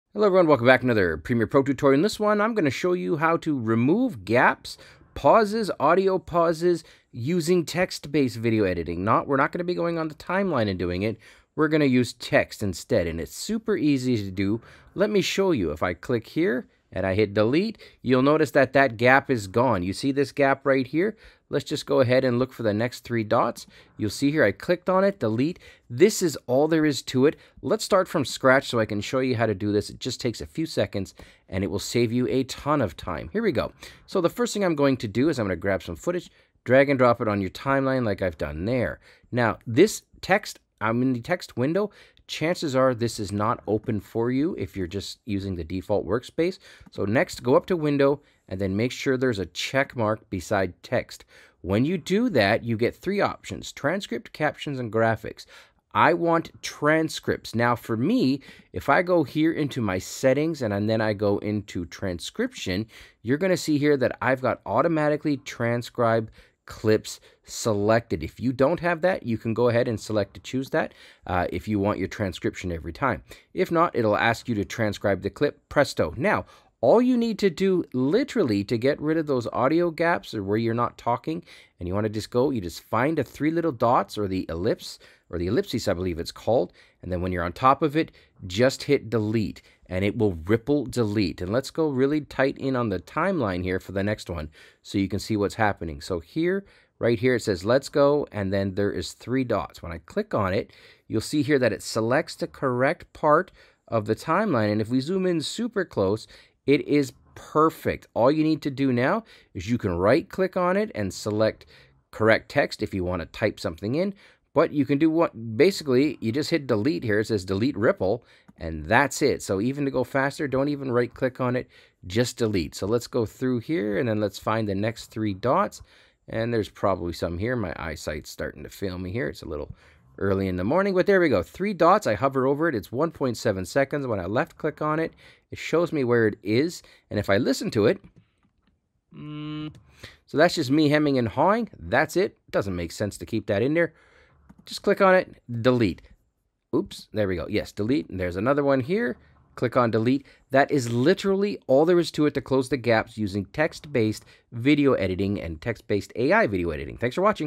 How To Remove Audio Gaps and Pauses in Premiere Pro 2024 - Text Based Editing
Welcome to another Adobe Premiere Pro tutorial! 🎬 In today's video, we are diving into the world of text-based video editing to show you a super easy and efficient way to remove those unwanted gaps and pauses in your audio. Say goodbye to tedious timeline adjustments and hello to a smoother, more professional video editing experience! 🚀

🕒 Timestamps:
00:00 - Introduction
00:19 - Quick Demonstration of Removing Gaps
00:33 - Starting from Scratch: Step-by-Step Guide
00:51 - Setting Up Your Workspace for Text Editing
01:13 - How to Automatically Transcribe Clips
01:31 - Finding and Removing Audio Gaps with Text
01:48 - Zooming in for Precision Editing
02:06 - Speeding Up Your Editing Workflow
02:20 - Real-Time Example and Conclusion

💻 What You'll Learn:

How to remove audio gaps and pauses using text-based video editing in Premiere Pro
Setting up your workspace for text editing
How to automatically transcribe your clips for easier editing
Tips and tricks to speed up your video editing workflow

🛠️ Tools and Resources:

Text window in Premiere Pro
Transcription settings